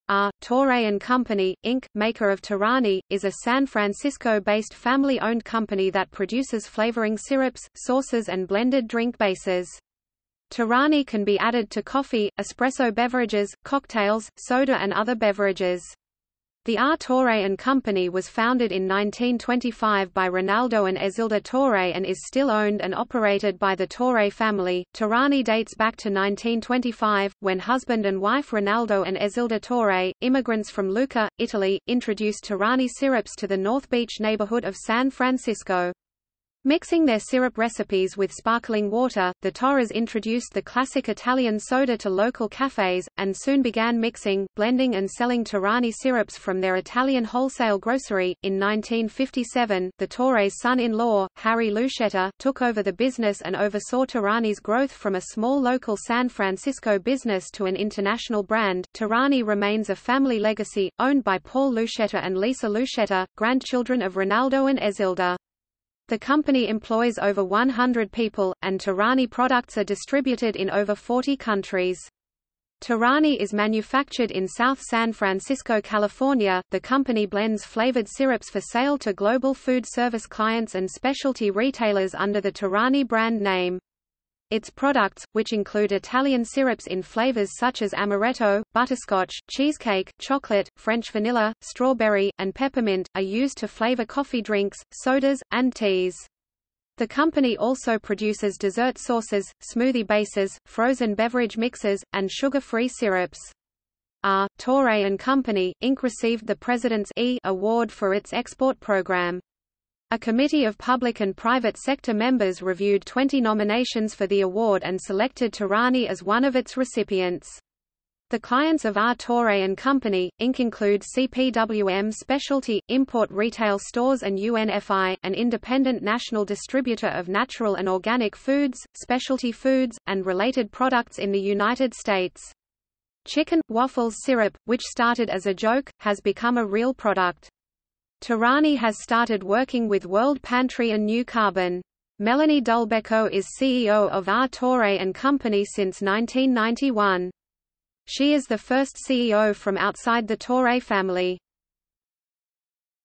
R. Torre & Company, Inc. | Wikipedia audio article
- Socrates

SUMMARY
R. Torre & Company, Inc., maker of Torani, is a San Francisco-based family-owned company that produces flavoring syrups, sauces and blended drink bases. Torani can be added to coffee, espresso beverages, cocktails, soda and other beverages. The R. Torre & Company was founded in 1925 by Rinaldo and Ezilda Torre and is still owned and operated by the Torre family.Torani dates back to 1925, when husband and wife Rinaldo and Ezilda Torre,  immigrants from Lucca, Italy,   introduced Torani syrups to the North Beach neighborhood of San Francisco. Mixing their syrup recipes with sparkling water, the Torres introduced the classic Italian soda to local cafes, and soon began mixing, blending and selling Torani Syrups from their Italian wholesale grocery.In 1957, the Torre's son-in-law, Harry Lucheta, took over the business and oversaw Torani's growth from a small local San Francisco business to an international brand.Torani remains a family legacy, owned by Paul Lucheta and Lisa Lucheta, grandchildren of Rinaldo and Ezilda. The company employs over 100 people, and Torani products are distributed in over 40 countries. Torani is manufactured in South San Francisco, California.The company blends flavored syrups for sale to global food service clients and specialty retailers under the Torani brand name. Its products, which include Italian syrups in flavors such as amaretto, butterscotch, cheesecake, chocolate, French vanilla, strawberry, and peppermint, are used to flavor coffee drinks, sodas, and teas. The company also produces dessert sauces, smoothie bases, frozen beverage mixes, and sugar-free syrups. R. Torre & Company, Inc. received the President's "E" Award for its export program. A committee of public and private sector  members reviewed 20 nominations for the award and selected Torani as one of its recipients. The clients of R. Torre & Company, Inc. include CPWM specialty/import retail stores and UNFI, an independent national distributor of natural and organic foods, specialty foods, and related products in the United States. Chicken 'N Waffles syrup—which started as a joke—has become a real product. Torani has started working with World Pantry and New Carbon. Melanie Dulbecco is CEO of R. Torre & Company since 1991. She is the first CEO from outside the Torre family.